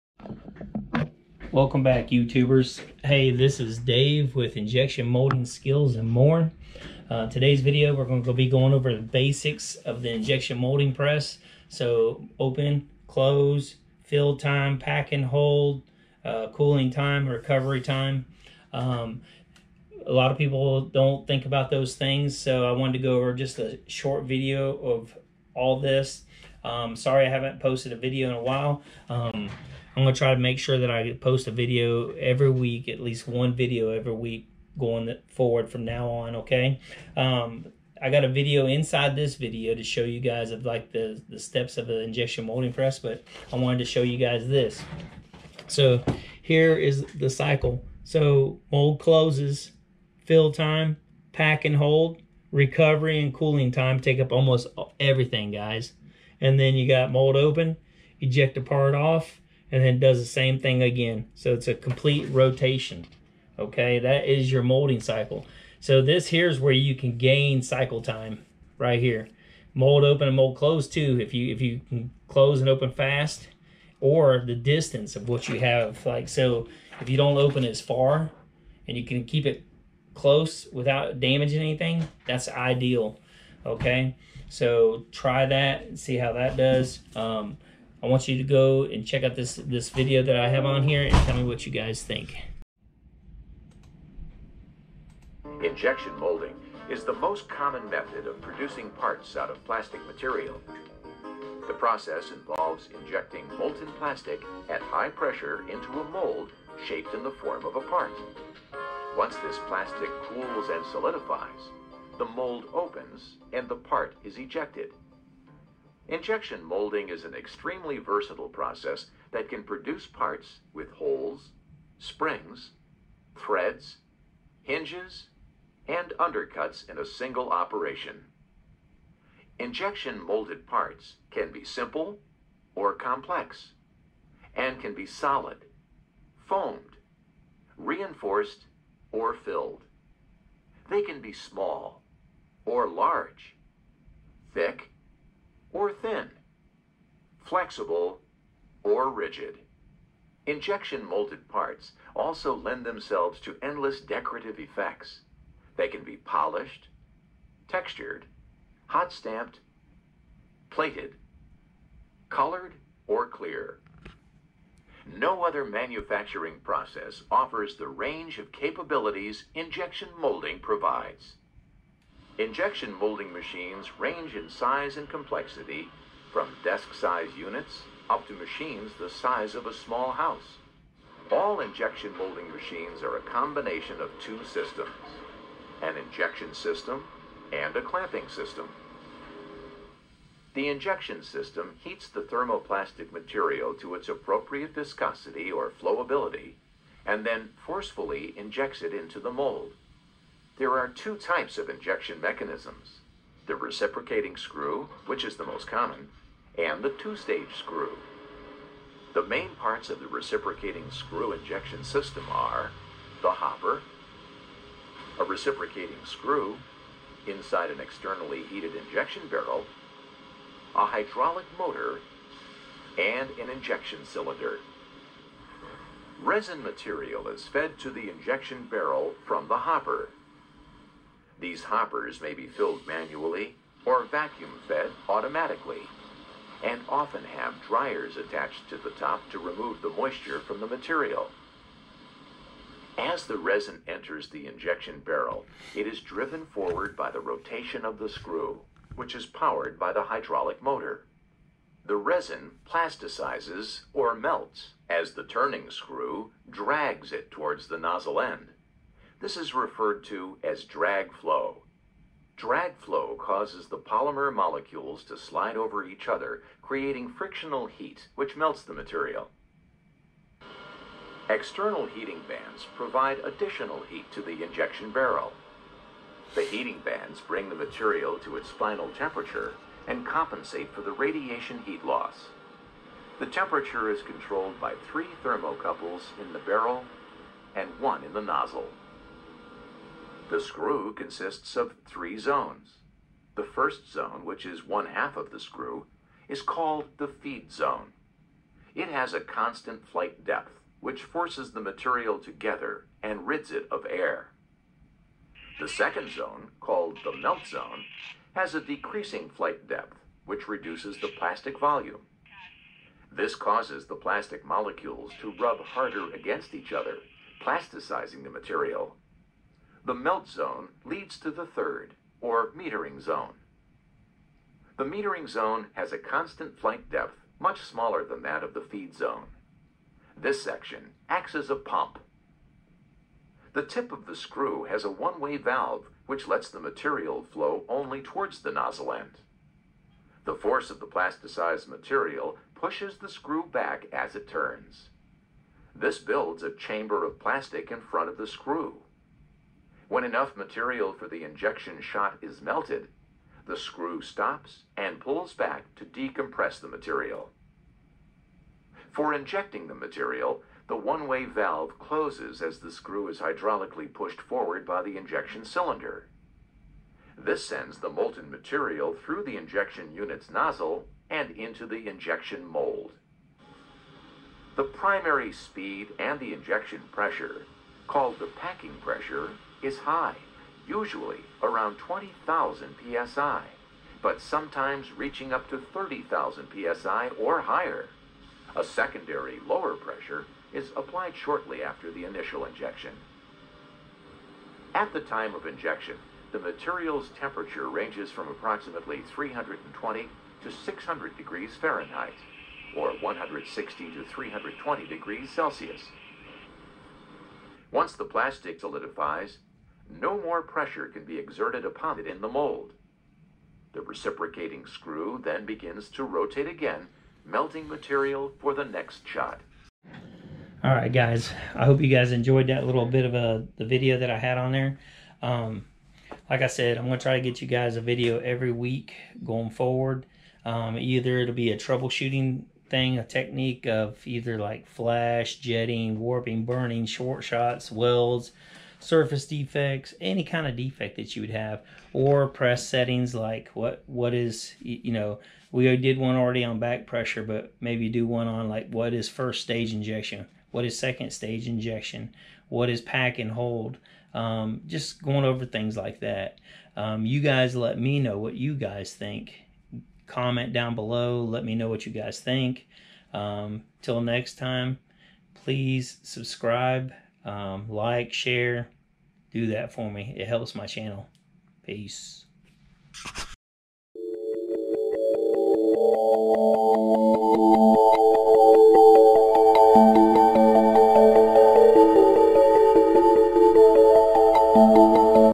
Basic Injection Molding Cycle Steps
The video goes over the steps of the molding cycle,
Clamp Close
Injection ( Fill Time )
Pack & Hold
Screw Recovery & Cooling Time
Clamp Open
Eject Part
Start Over
Let me know if you have any ideas on the next video if you are interested.
Thanks for the support & motivation to push forward.